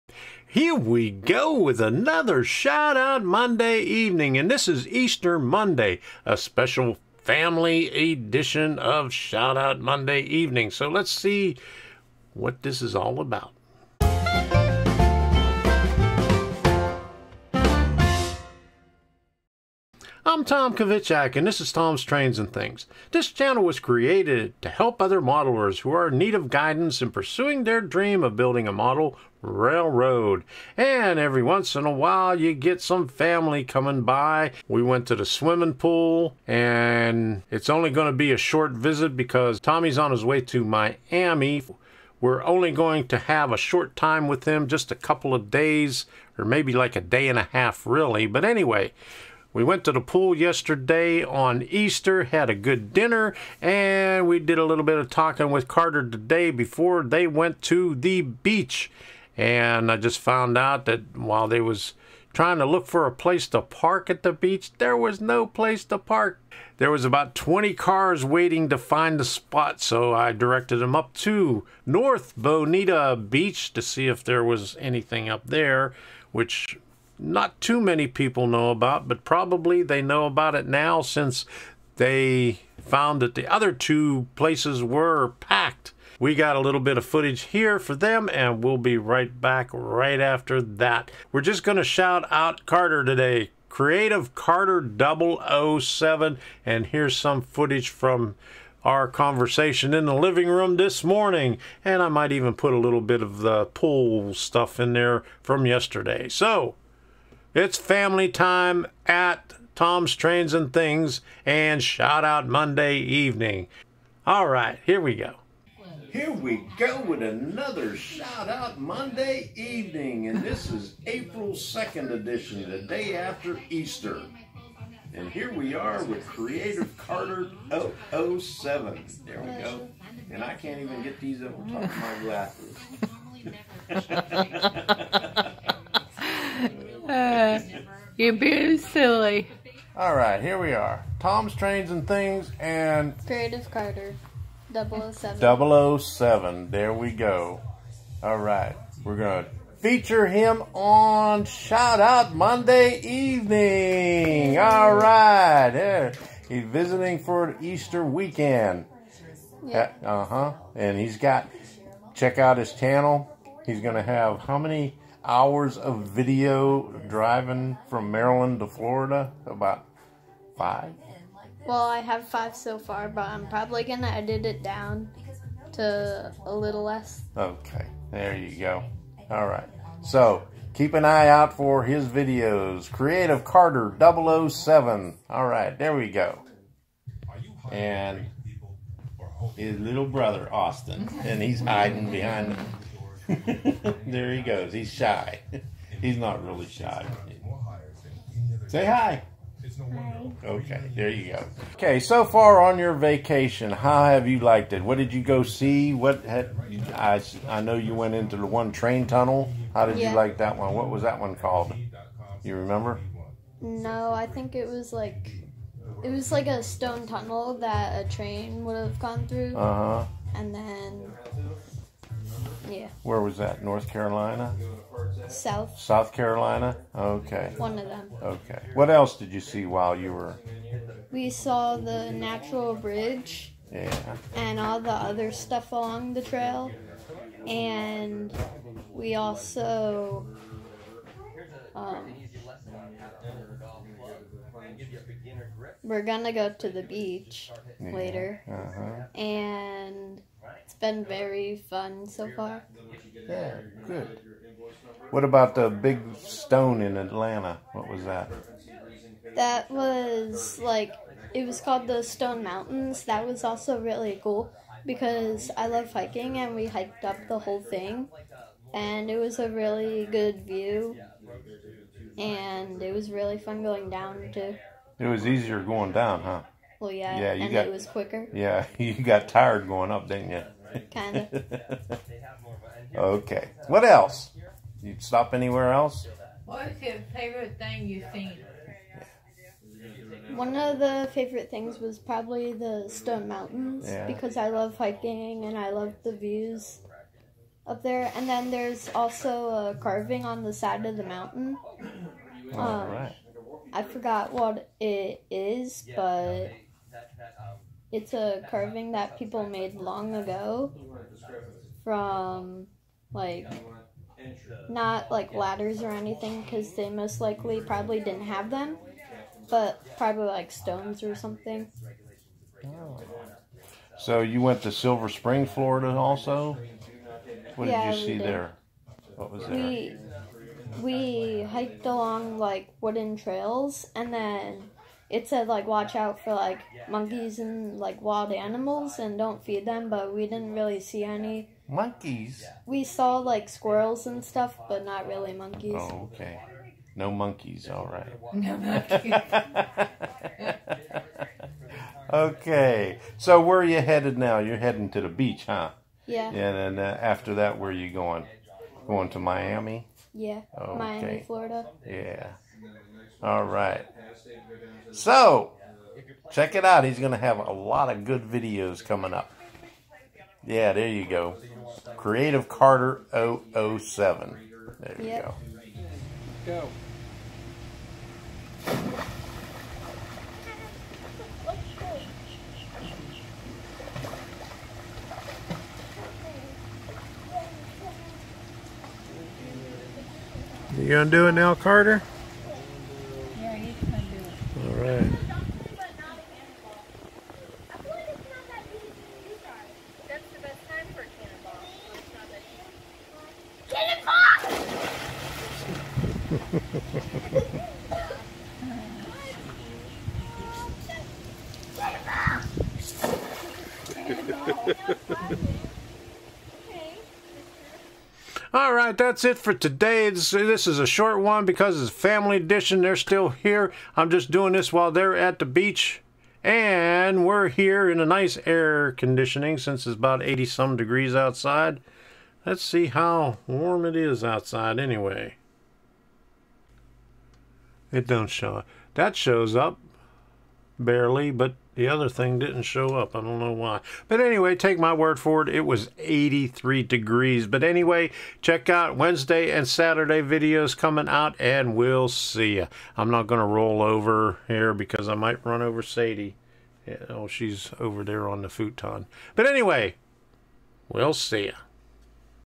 Shout Out Monday Evening - Family Edition
With the Easter Holiday and family visiting for the weekend, we are having a Shout Out Monday Evening - Family Edition. We have another YouTuber in the family - Creative Carter007.

If you would like to help with translating this or any other video of mine, click this

S U P P O R T  THIS  C H A N N E L